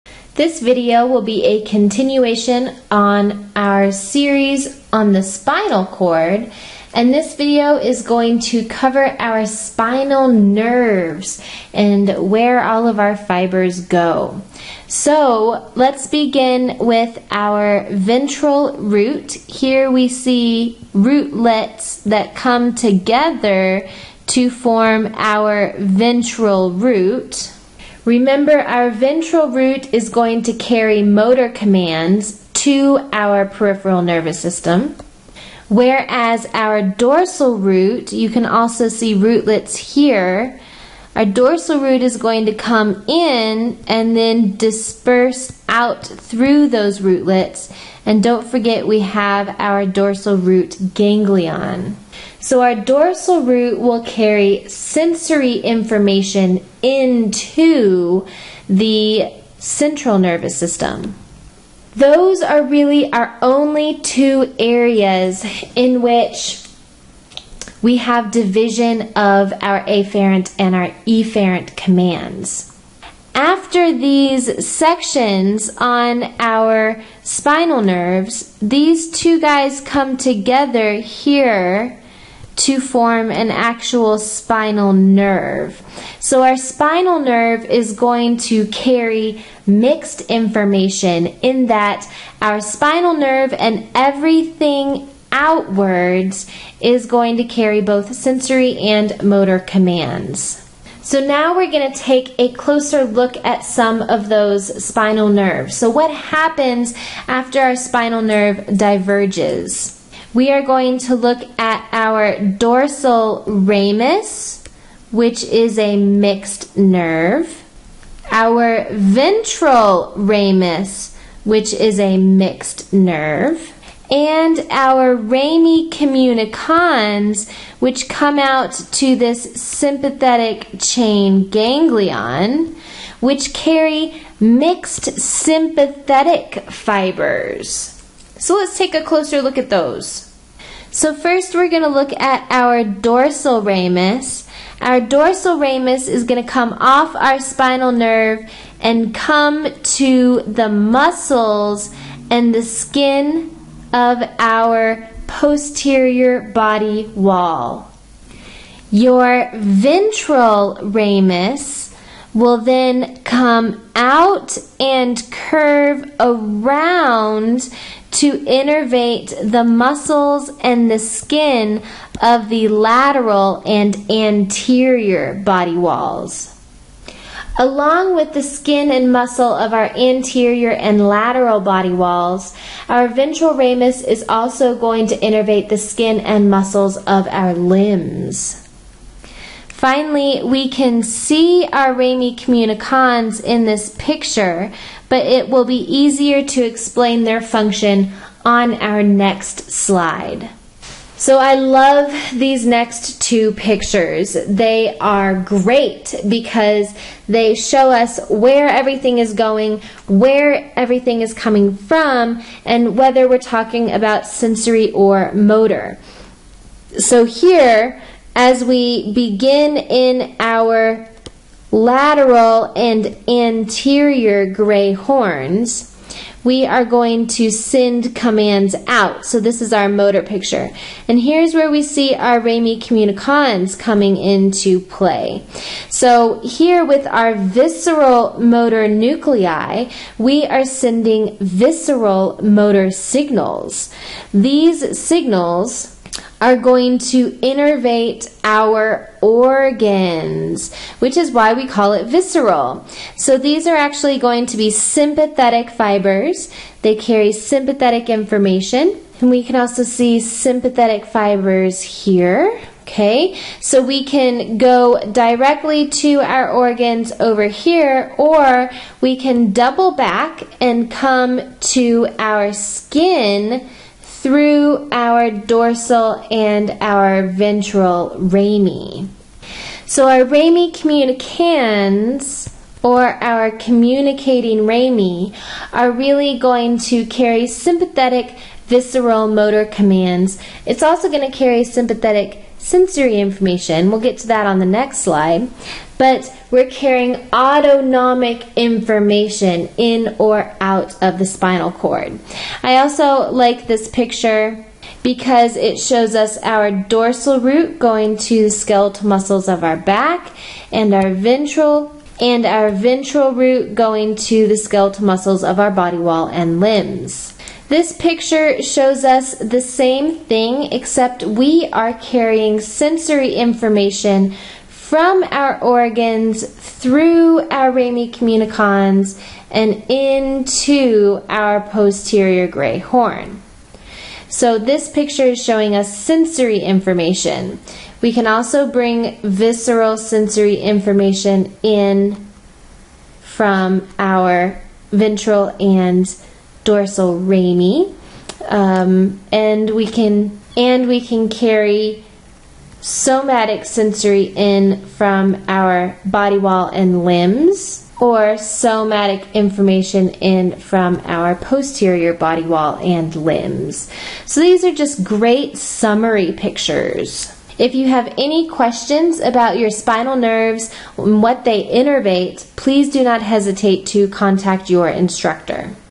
Spinal Nerves
This video gives a brief description of the spinal nerves that connect to the spinal cord: ventral and dorsal roots, the dorsal ramus, ventral ramus, and rami communicantes (communicating rami).